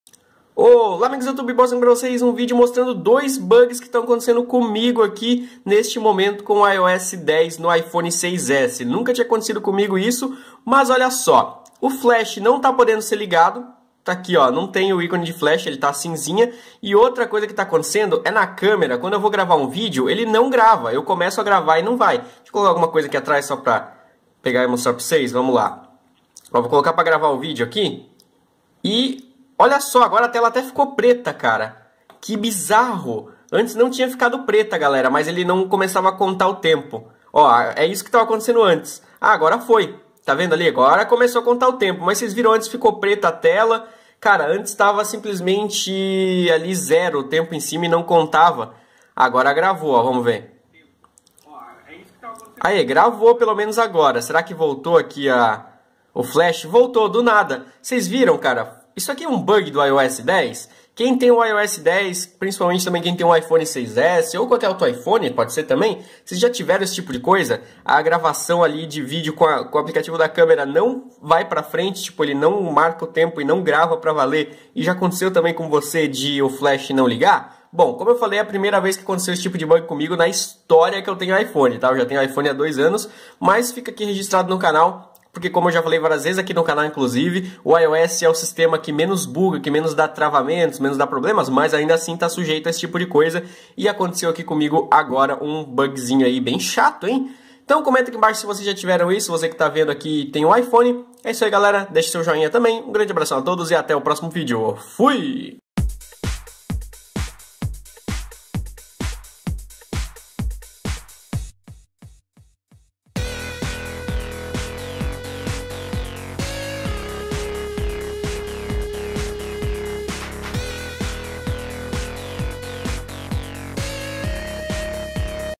Bug da Câmera e Flash no iOS 10 - iPhone 6S
Mostrando dois bugs que aconteceram com o meu iPhone 6S atualizado no iOS 10.2 recentemente.